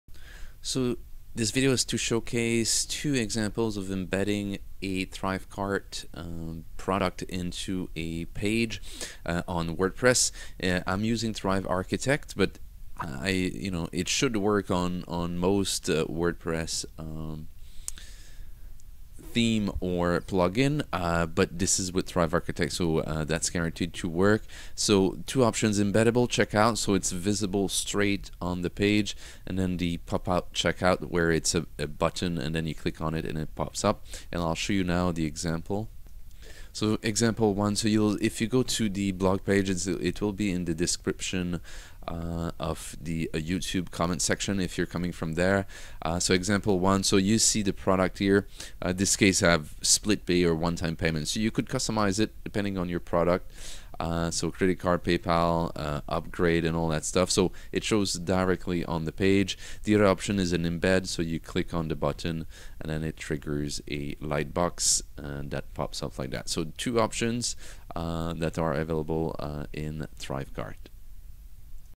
ThriveCart with Thrive Architect/Thrive Themes
2 options for Embedding ThriveCart with Thrive Architect/Thrive Themes